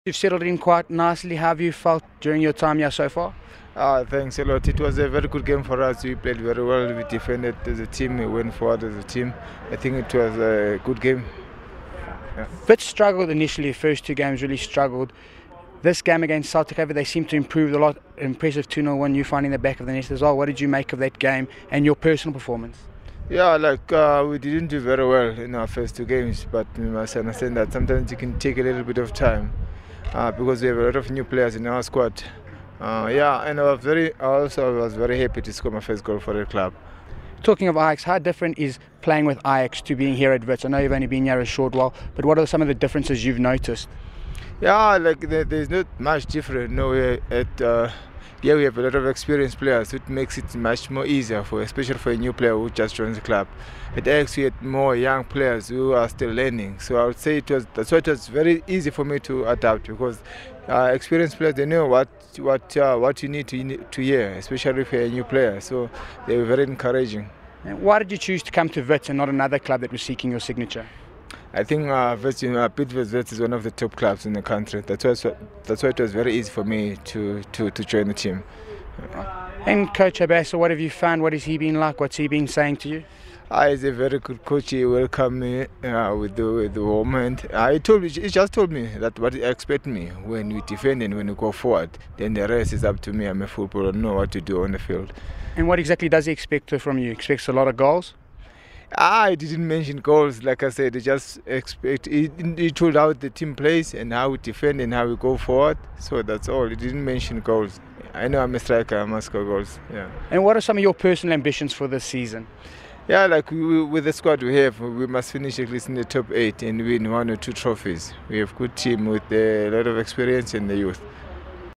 Thembinkosi Fanteni interview
Terror Fanteni chats after his debut goal for Bidvest Wits - and insists he made the perfect career choice.